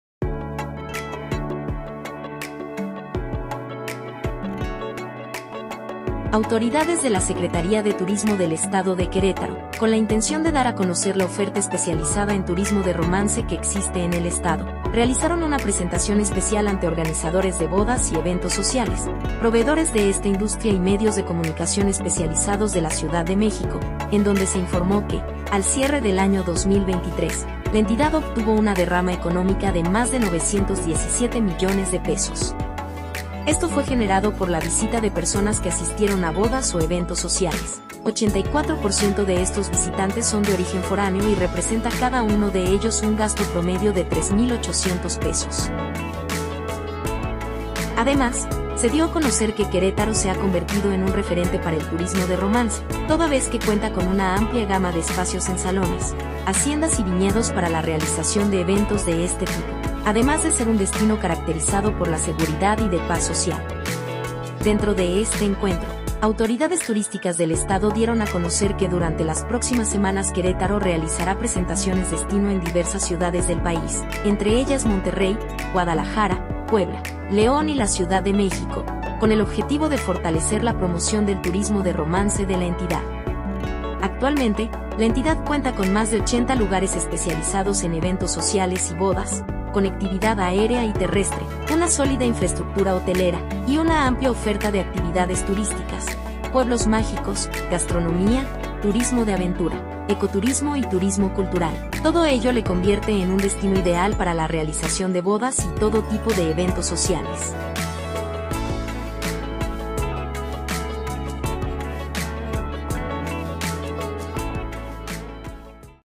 Turismo de romance en Querétaro generó 917 MDP
Informativo @TuristikTV

Esta es una producción de TuristikTV para Grupo Origen.